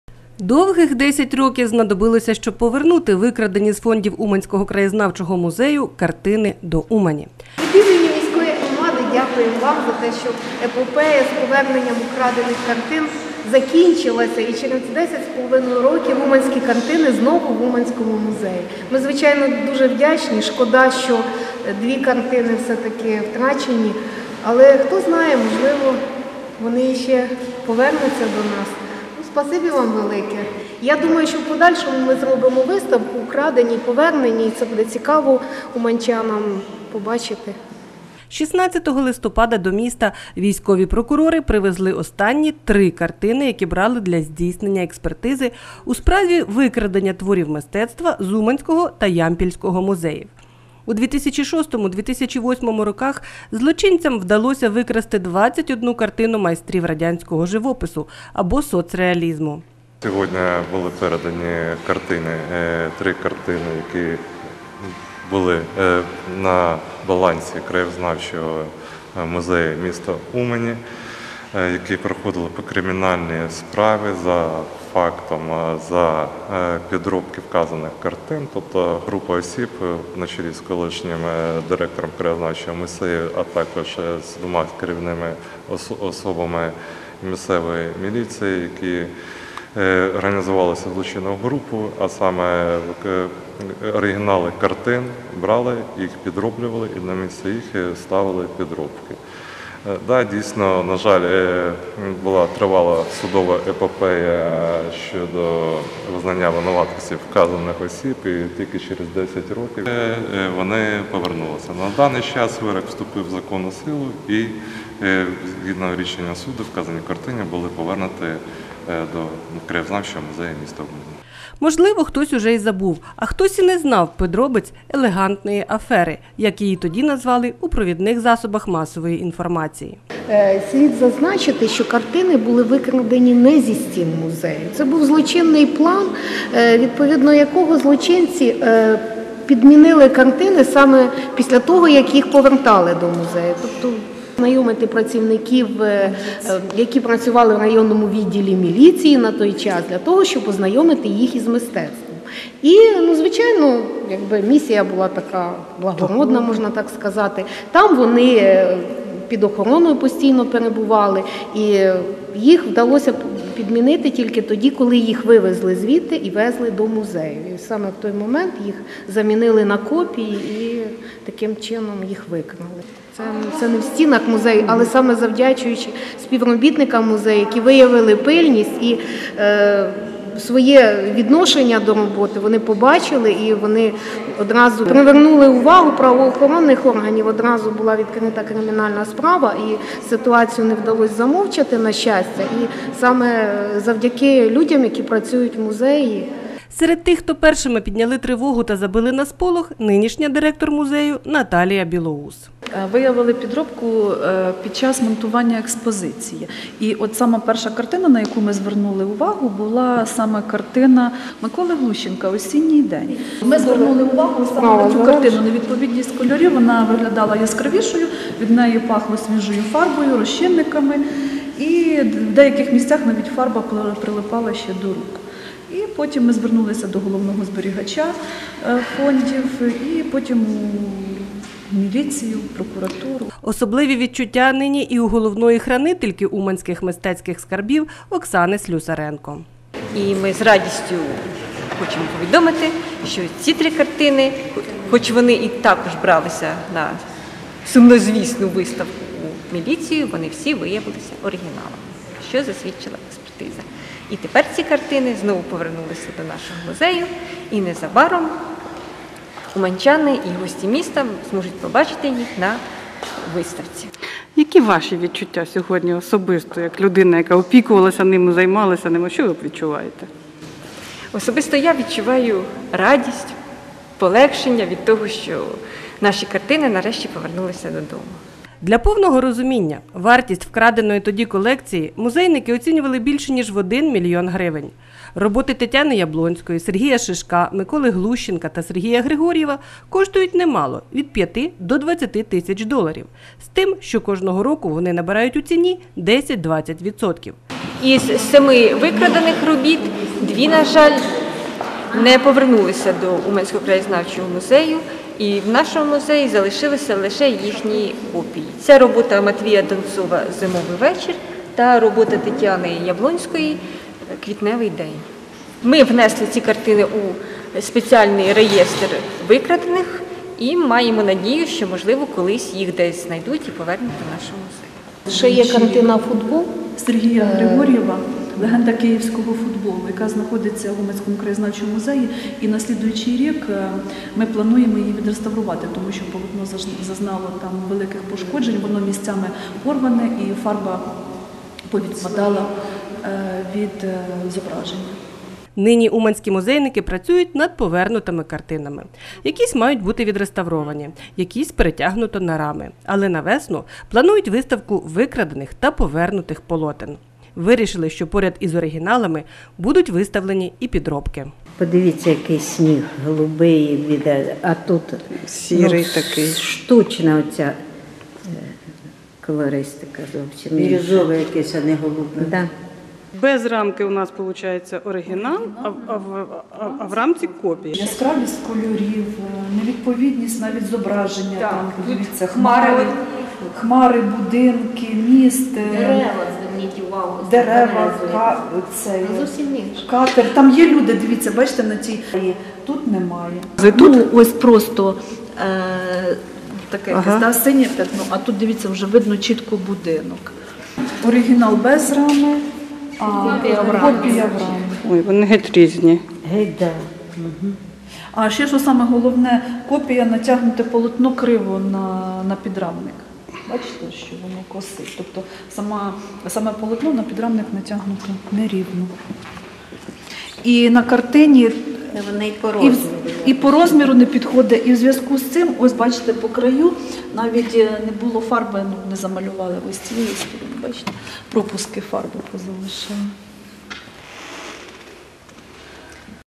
Фінал «елегантної афери»
Більше десяти років знадобилося правоохоронцям, щоб повернути до Умані картини майстрів соцреалізму, викрадені з фондів Уманського краєзнавчого музею. Навесні уманчани побачать оригінали і копії викрадених картин, зможуть знайти «100 відмінностей».